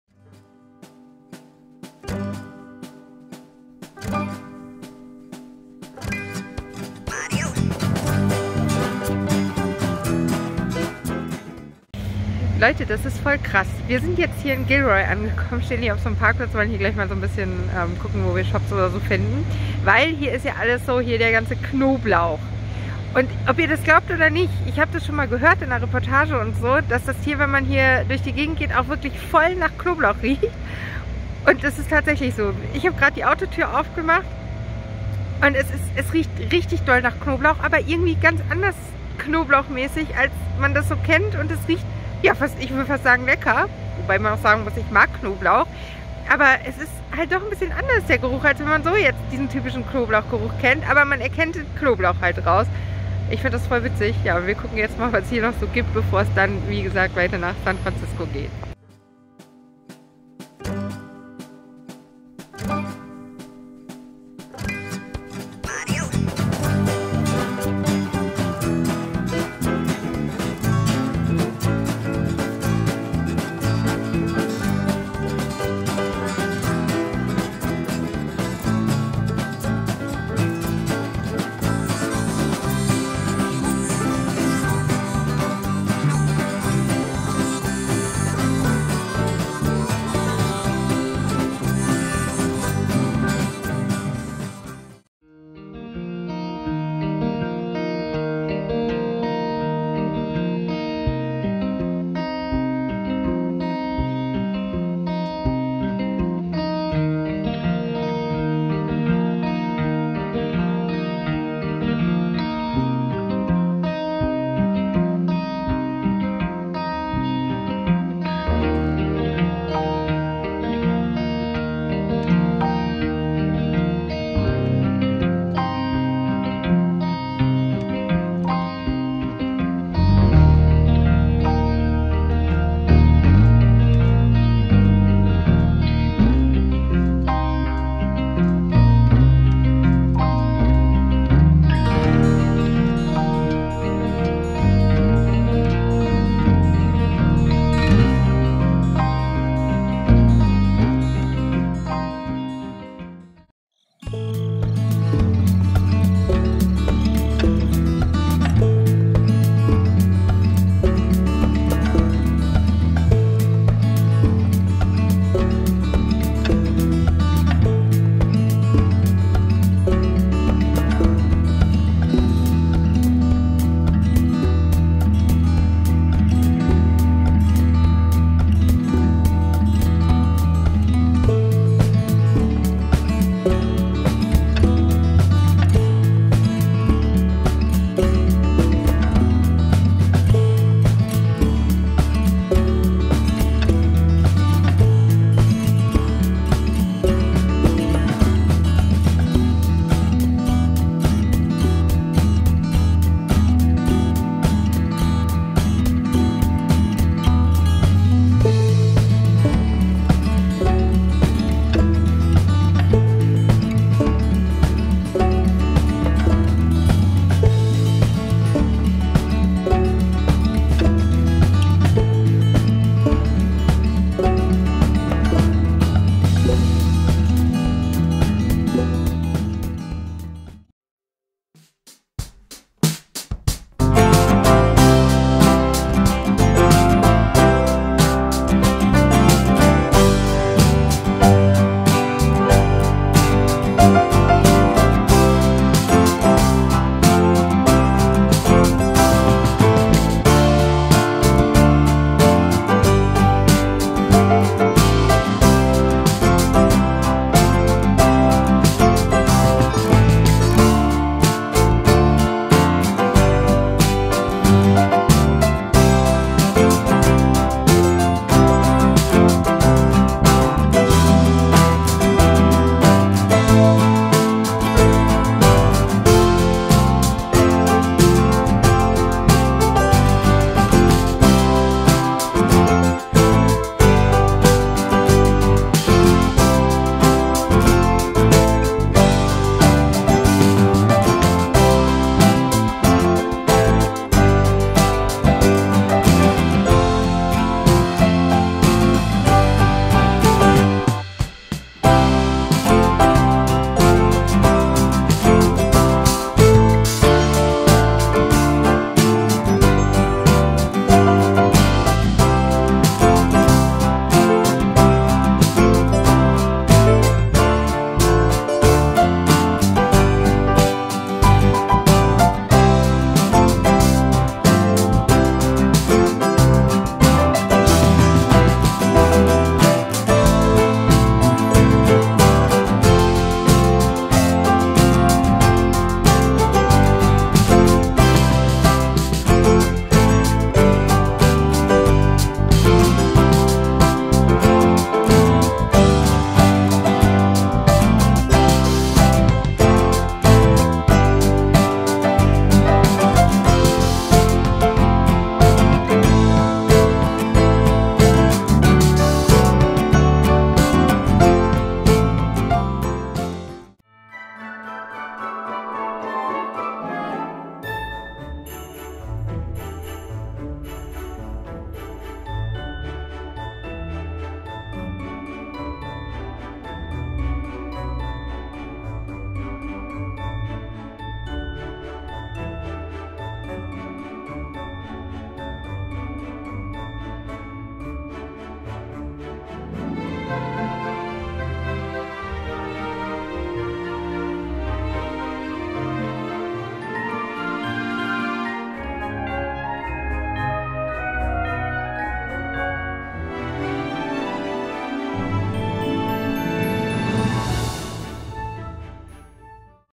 Vlog 9: Erst Knoblauch, dann San Francisco
Unsere letzte Etappe auf dem kleinen Roadtrip führte uns zuerst in die Stadt des Knoblauchs: Gilroy. Anschließend ging es dann dem Endziel dieser Reise entgegen: San Francisco. Dort ging es natürlich direkt auf erste Sightseeingtour.